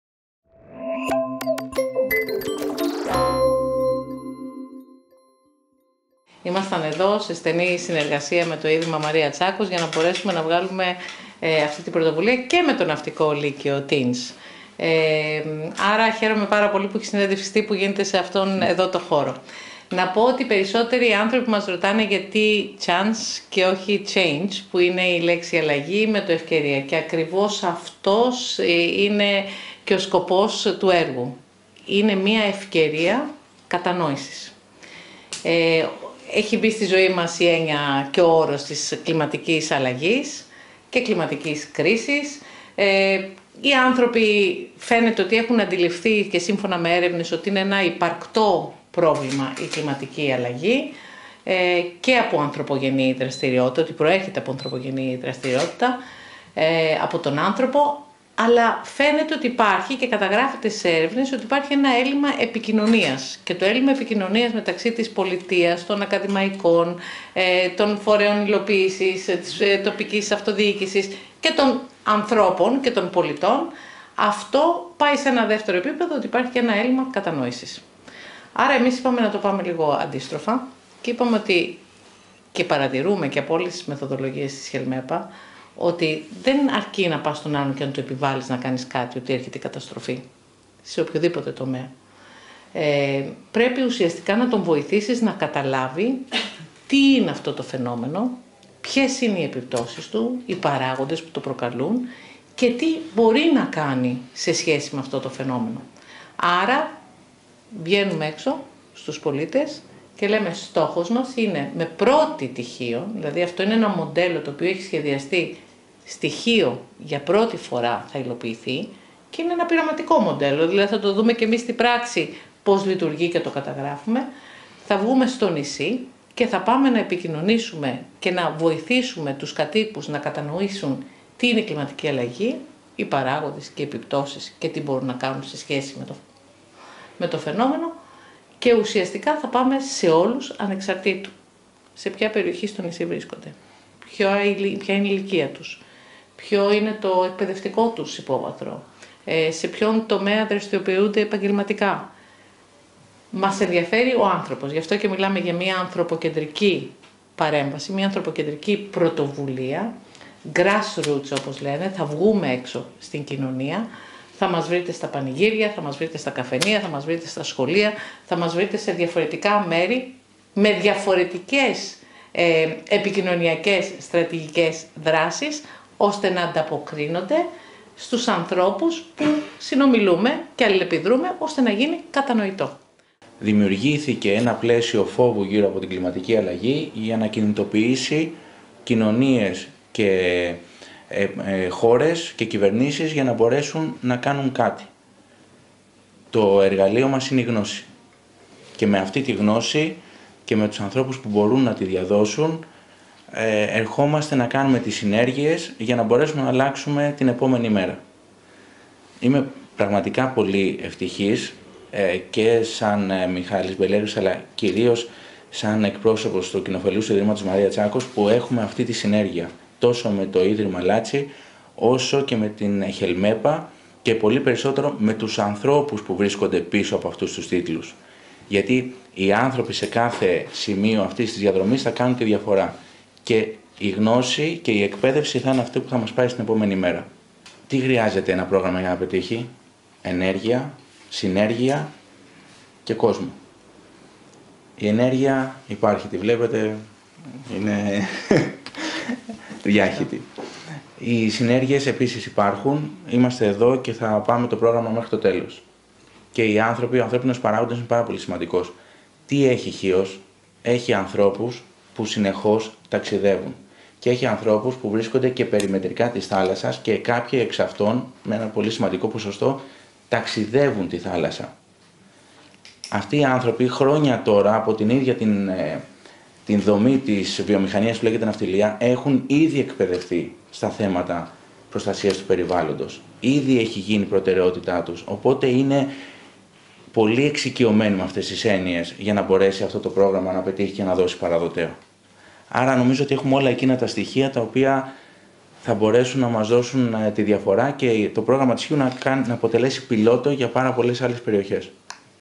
Ξεκινά από τη Χίο πιλοτικό πρόγραμμα δράσης για την κλιματική αλλαγή
Παρουσία του Υπουργού Κλιματικής Κρίσης και Πολιτικής Προστασίας, Βασίλη Κικίλια ο οποίος θα απευθύνει και σχετικό χαιρετισμό θα ξεκινήσει αύριο, στις 12 το μεσημέρι τις εργασίες του στο Ομήρειο, συνέδριο για την κλιματική αλλαγή. Διοργανώνεται από το Ίδρυμα «Μαρία Τσάκος" σε συνεργασία το Ίδρυμα Ιωάννη Σ. Λάτση και την HELMEPA. Την κεντρική ομιλία θα εκφωνήσει ο Πρόεδρος του Ιδρύματος «Μαρία Τσάκος» Ευθύμιος Μητρόπουλος. Το συνέδριο θα τιμήσει με την παρουσία του και θα χαιρετίσει ο Καπετάν Παναγιώτης Ν. Τσάκος.
«Σε αυτό το πρόγραμμα συμμετοχή και λόγο έχουν όλοι οι κάτοικοι της Χίου ανεξαρτήτως ηλικίας,  επαγγέλματος και μορφωτικού επιπέδου, »ανέφερε η  Όλγα Σταυροπούλου, γενική διευθύντρια της HELΜΕPA στο πλαίσιο συνέντευξης τύπου.